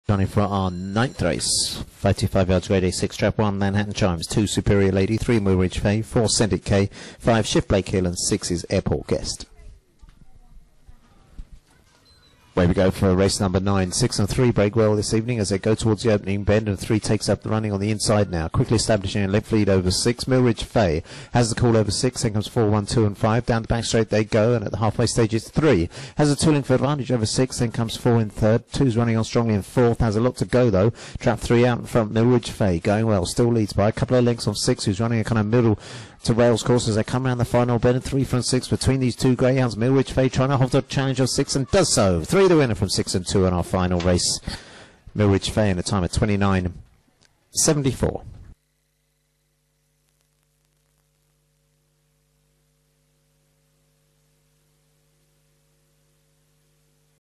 IGB - Hit Or Bust 25/08/2018 Race 9 - Tralee
Stadium: Hit Or Bust
Track Name: Tralee
Date: 25/08/2018
Hour: 21:44

Results

1st    3     MILLRIDGE FAY       2/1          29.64
2nd    6     AIRPORT GUEST       4/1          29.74
3rd    2     SUPERIOR LADY       5/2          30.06
4th    4     SEND IT KAY         2/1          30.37
5th    5     SHIFT BLAKE HILL    3/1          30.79
6th    1     MANHATTAN CHIMES    2/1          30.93

|TRL_2018_08_25_09